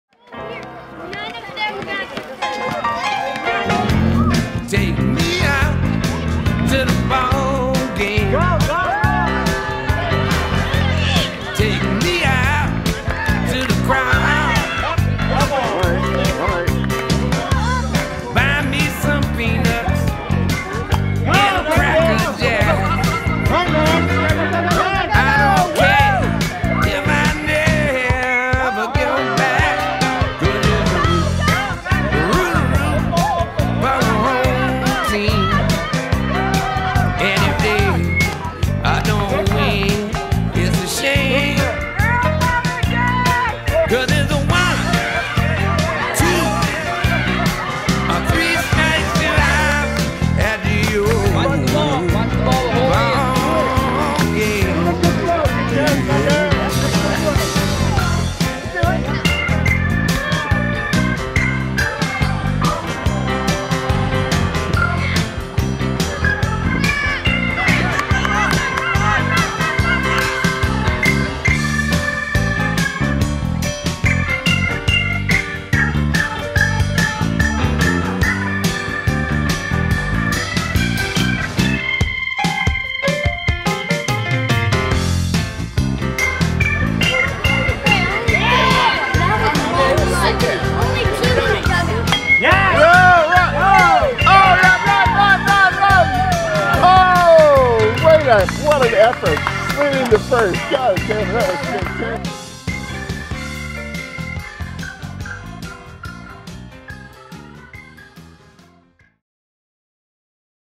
Play Ball!
This video is about the Maggione brothers, Desmond and Owen, rocking the diamond in Philly.